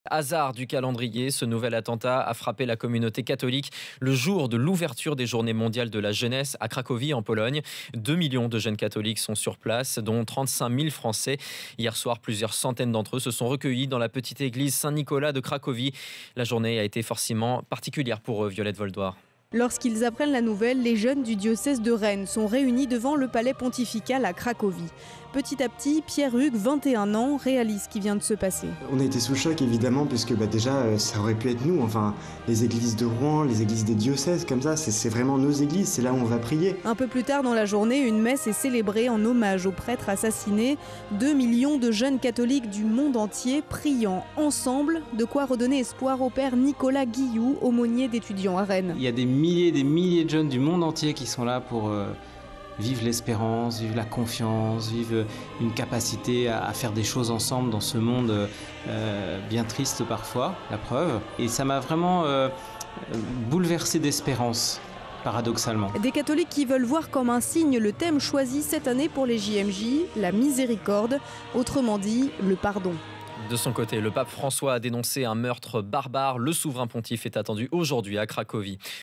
Aux JMJ, "on est mis au pied du mur sur notre capacité à être miséricordieux"
Les jeunes catholiques ont envahi les rues de Cracovie en Pologne pour les Journées mondiales de la jeunesse. Pour les jeunes pélerins français, ces journées vont avoir une résonnance particulière.